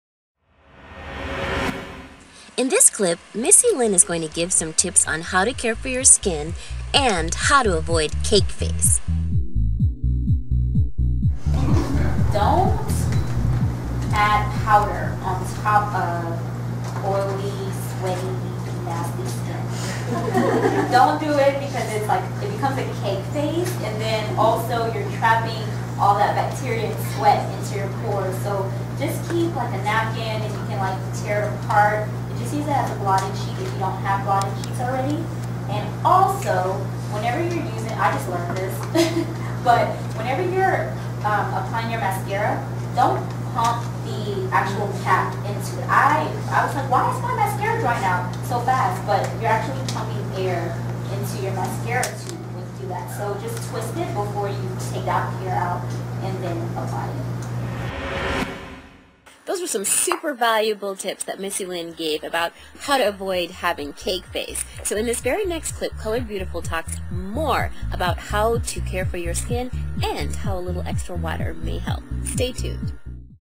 Pt.3 of 5 "Hottie" Schatar's Digital Beauty Panel Video Special
Here is an up close look featuring digital stars Weylie, Evelina Barry, Jen Chae, Missy Lynn and Coloured Beautiful in my 5 part video special, "Leading Ladies of Digital Beauty." Get make-up tips, beauty pointers, and an insider's look at how these powerhouses built their popular digital beauty brands.
Schatar "Hottie" Schatar

Digital Beauty 5 Part Video Series - Part 3 of 5
Hosted, Produced, and Edited By
Schatar Sapphira Collier
Assistant Director Maxie J. Collier
Additional Images and Audio
Provided By VideoBlocks